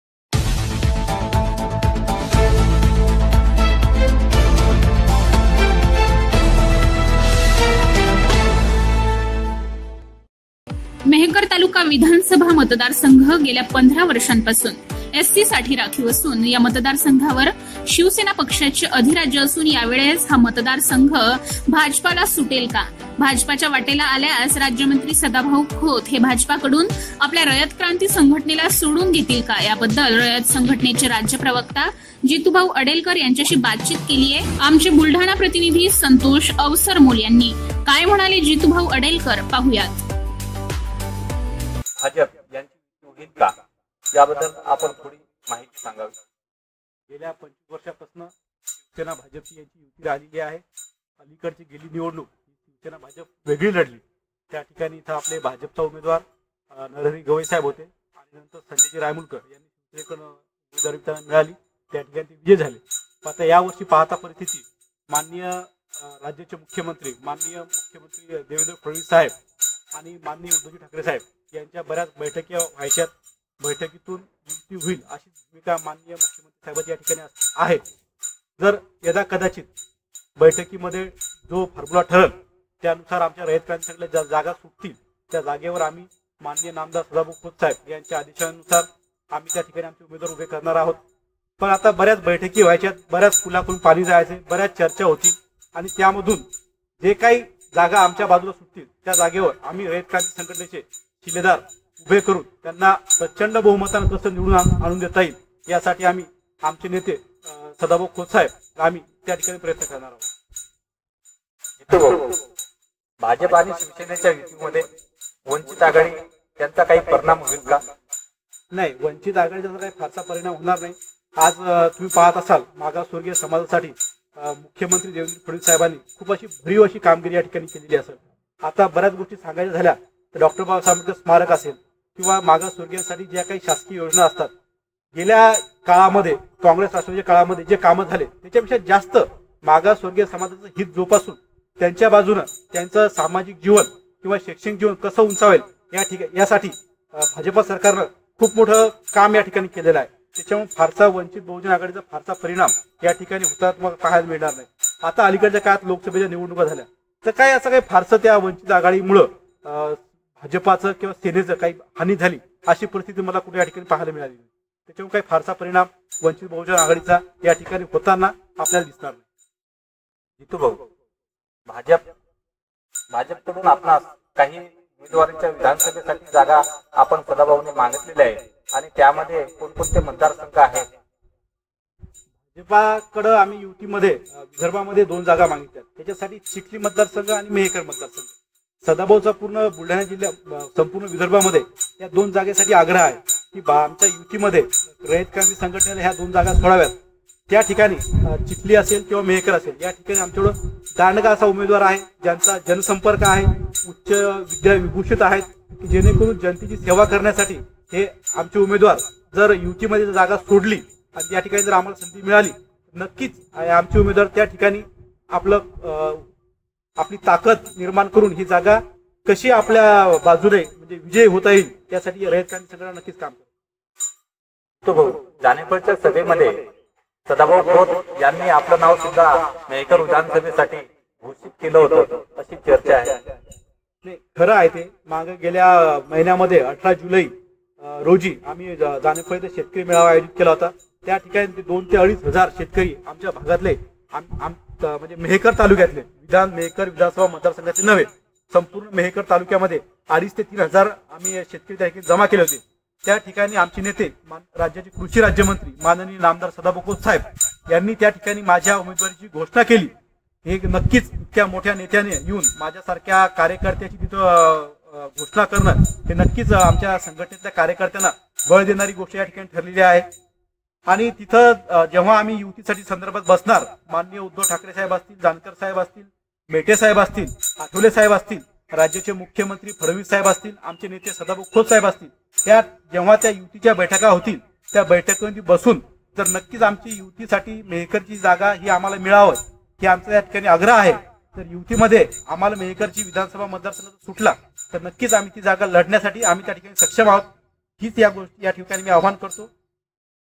मेहकर विधानसभा मतदार संघ भाजपा च्या वाट्याला येईल का ?
मेहकर तालुका विधानसभा मतदार संघ गेल्या पंधरा वर्षा पासुन SC साठी राखीव असुन,या मतदार संघावर शिवसेना पक्षाचे अधीराज्य असुन,यावेळेस भाजप पक्षा हा मतदार संघ सुटेल का ! भाजपा च्या वाट्याला आल्यास , राज्यमंत्री सदाभाऊ खोत ,हे भाजपा कडुन आपल्या रयत क्रांती संघटनेला सोडुन घेतील का ! या बद्ल रयत संघटनेचे राज्य प्रवक्ता जितुभाऊ अडेलकर यांची मुलाकात घेताना संतोष अवसरमोल बुलढाणा जिल्हा